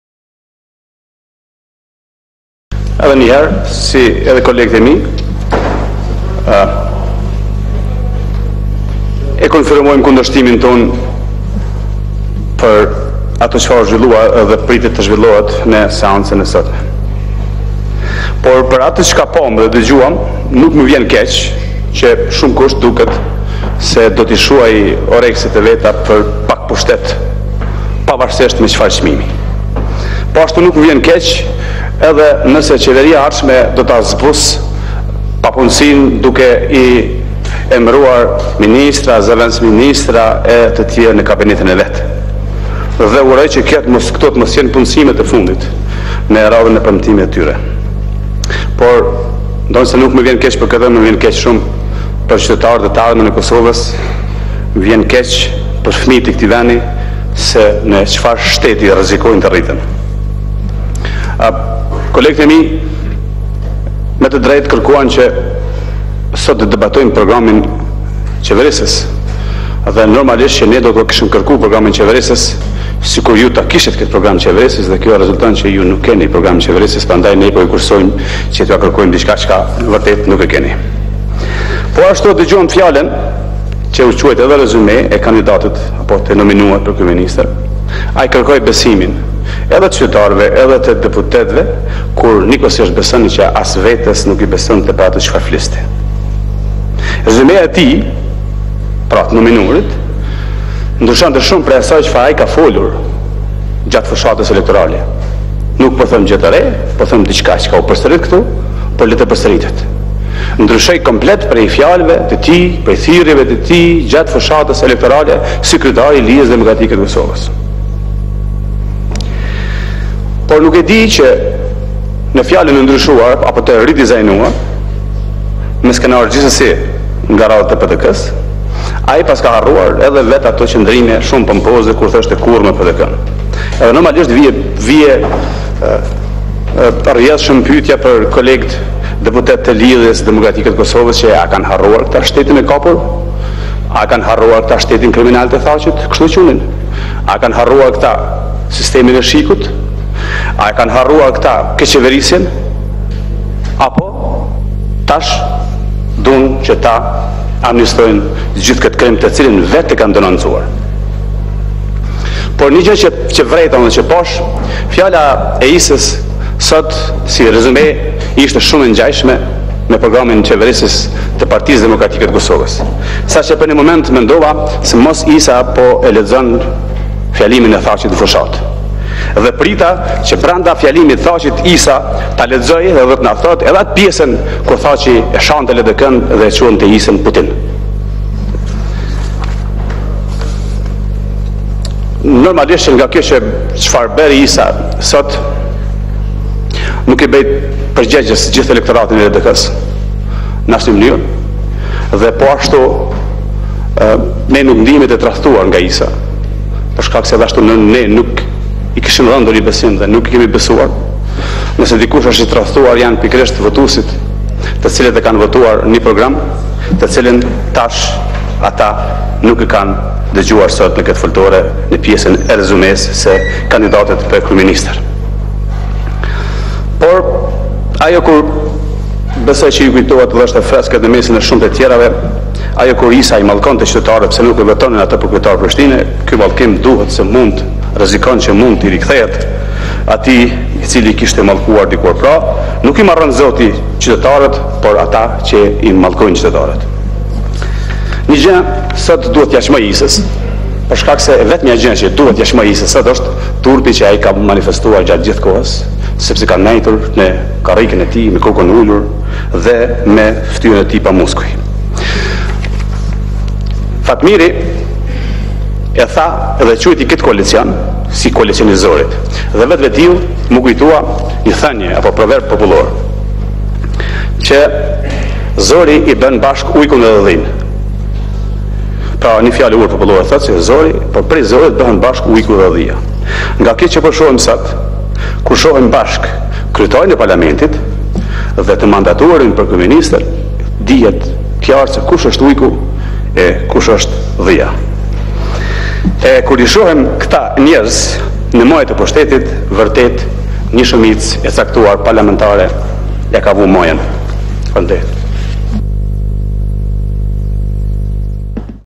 Rexhep Selimi: LDK-ja amnistoi krimin që dikur e ka denoncuar
8 dhjetor 2014